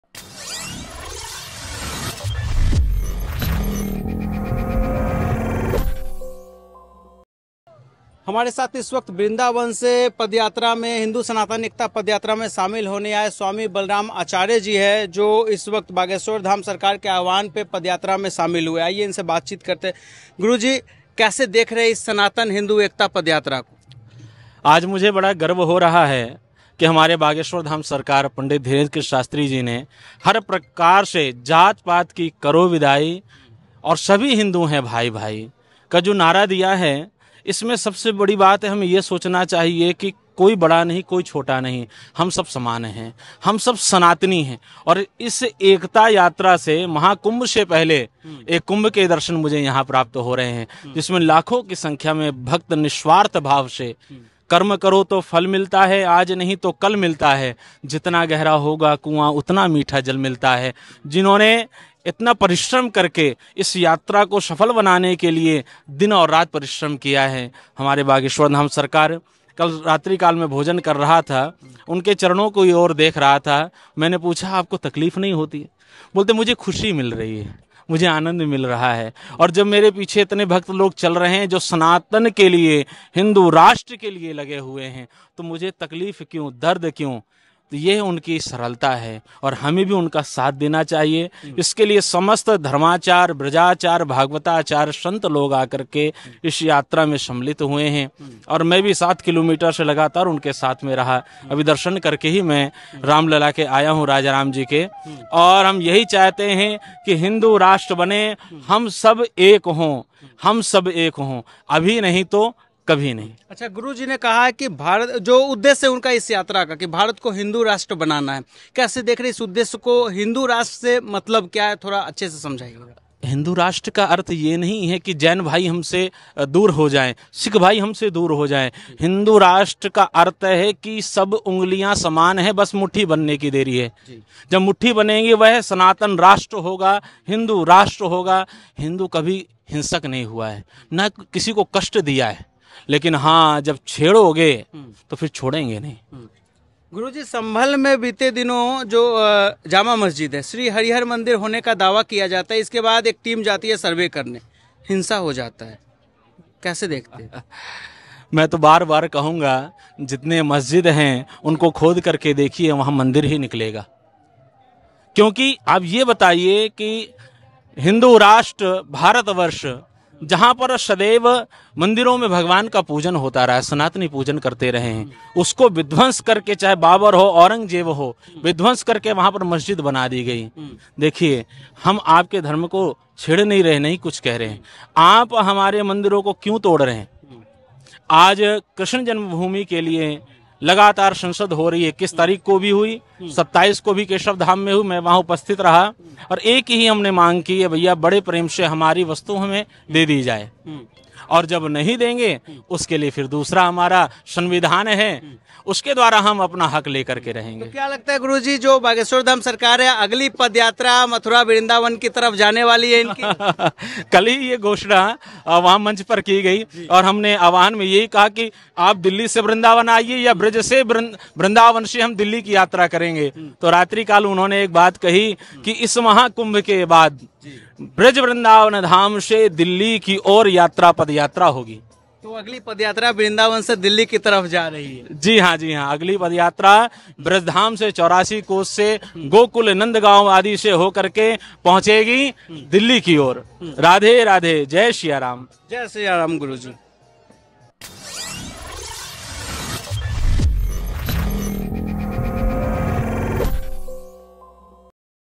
Sambhal कांड: Acharya Balram की ललकार- मस्जिदों को खोद कर देखो वहां मंदिर ही निकलेगा !
बागेश्वर धाम के महंत धीरेंद्र शास्त्री की सनातन एकता पदयात्रा में शामिल हुए Mathura के आचार्य बलराम महाराज का संभल कांड पर ऐलान- मस्जिदों को खोद कर देखो वहां मंदिर ही निकलेगा !

Jogi
Dheerendra